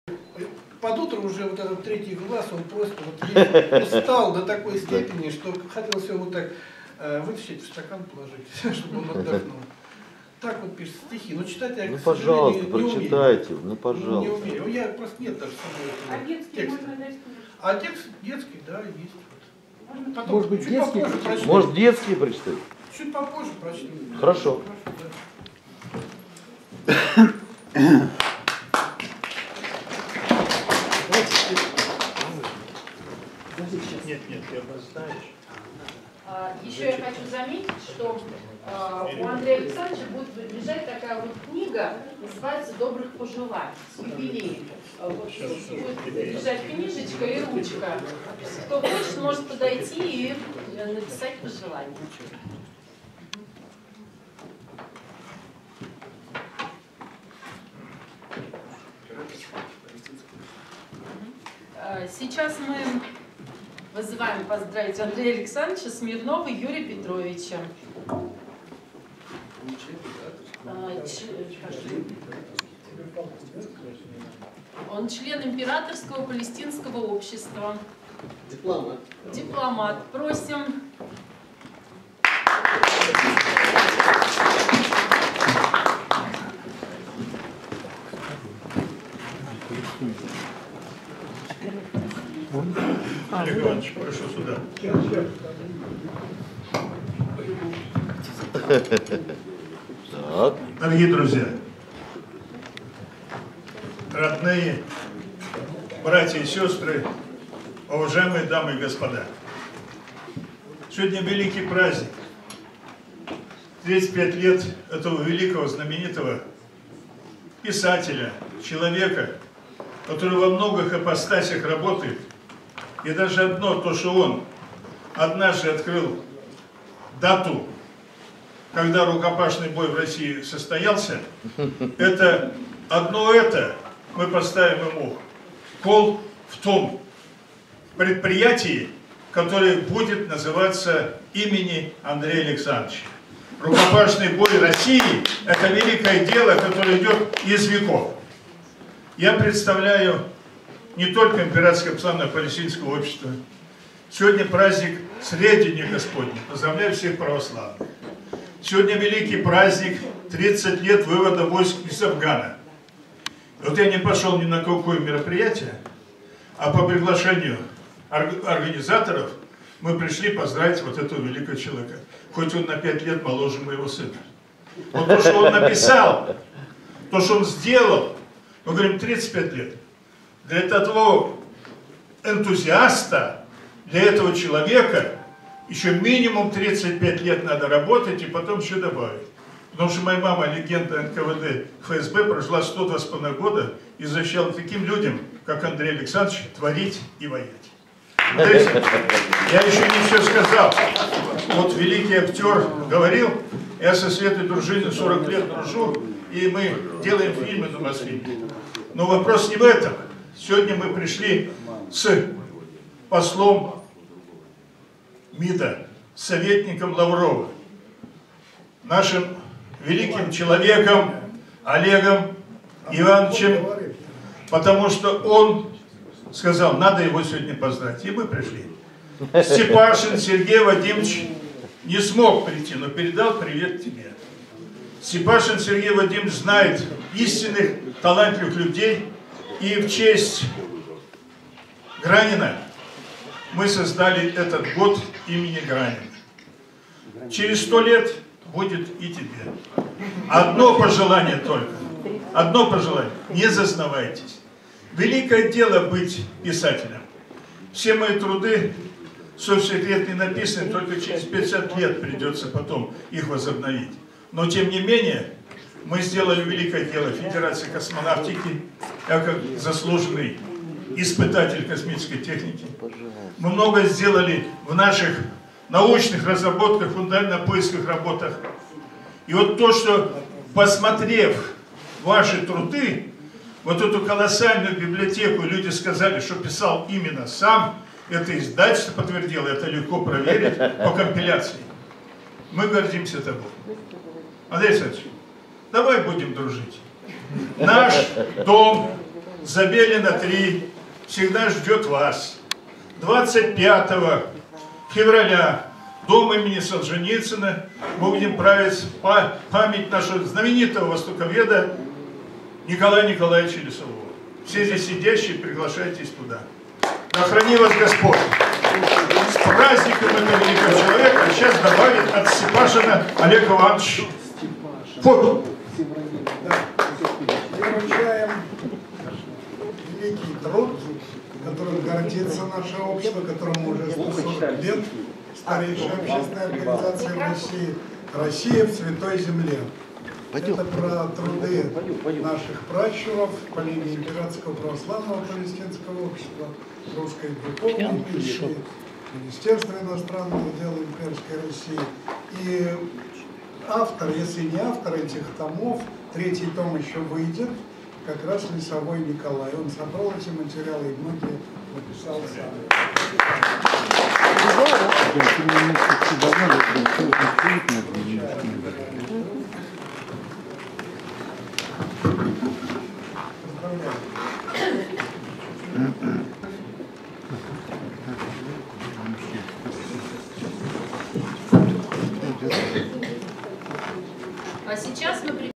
Тв. вечер Андрея Тюняева в ЦДЛ 15.02.2019
15 февраля 2019 г.
Москва, Центральный Дом писателей.
Творческий вечер Андрея Тюняева.
Поздравление друзей, коллег, поклонников таланта литератора.